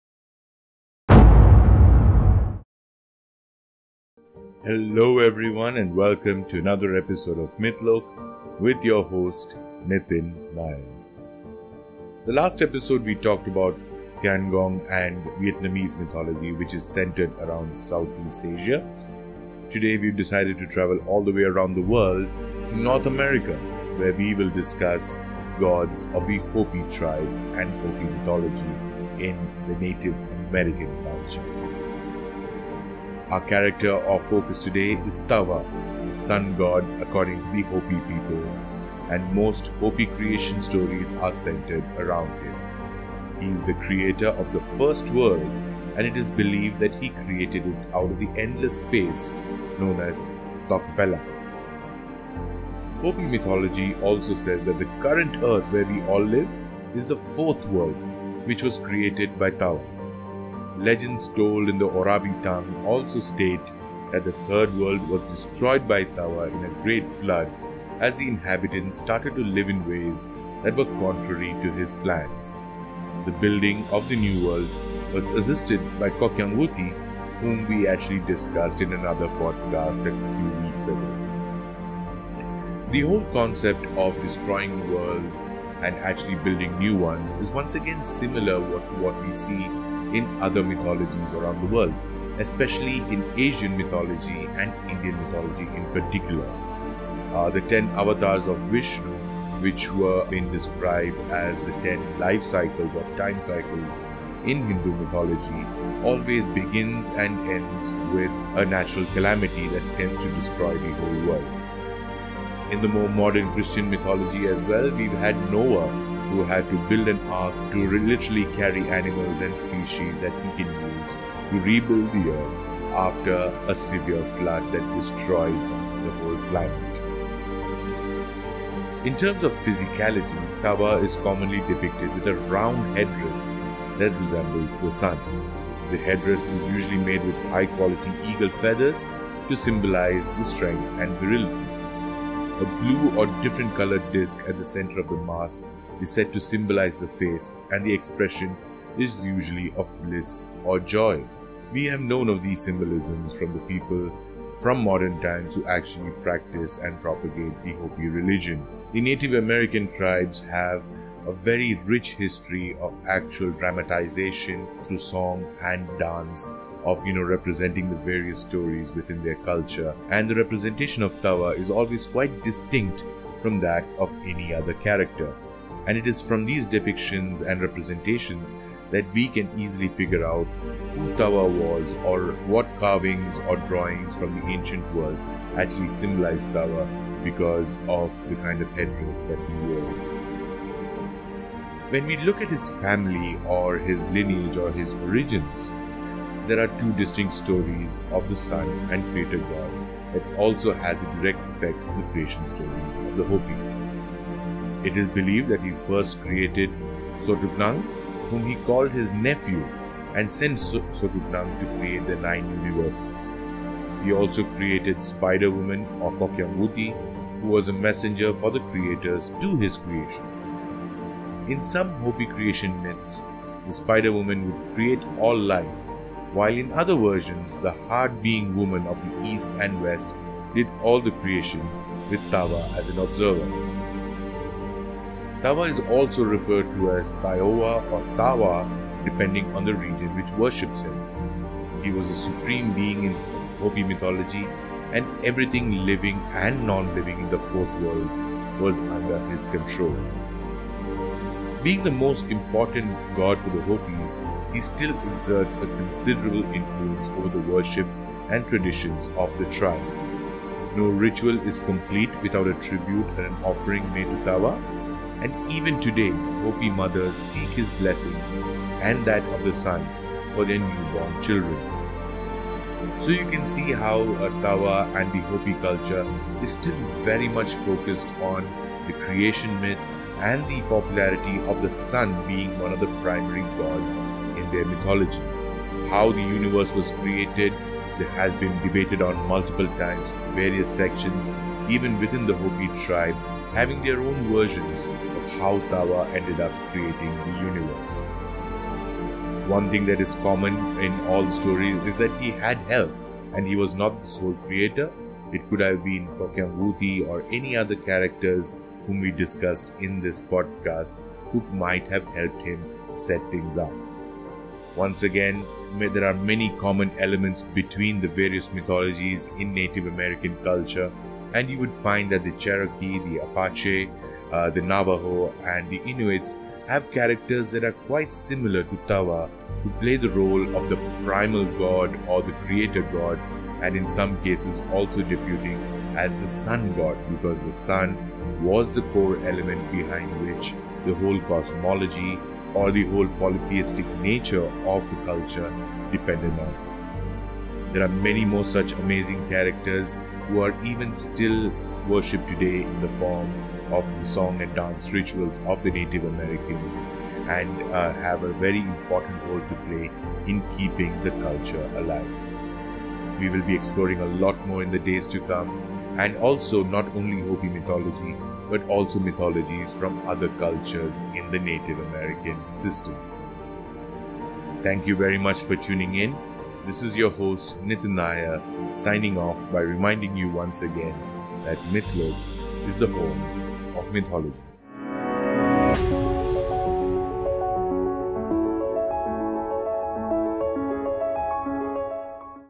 Tawa : The Sun God | Hopi Mythology | NativeAmerican Mythology | Mythlok
Tawa is the Sun god according to Hopi mythology and most Hopi creation stories are centred around him. He is the creator of the First World, and it is believed that he created it out of the endless space known as Tokpella.

Hopi mythology also says that the current earth where we all live is the Fourth World which was created by Tawa. Legends told in the Orabi tongue also state that the Third World was destroyed by Tawa in a great flood as the inhabitants started to live in ways that were contrary to his plans. The building of the new world was assisted by Kokyangwuti.

Tawa is commonly depicted with a round headdress that resembles the sun. The headdress is usually made with high quality eagle feathers to symbolise the strength and virility.

A blue or different coloured disk in the centre of the mask is said to symbolise the face and the expression is usually of bliss or joy.

This is where there are two distinct stories of the sun and creator god that also has a direct effect on the creation stories of the Hopi.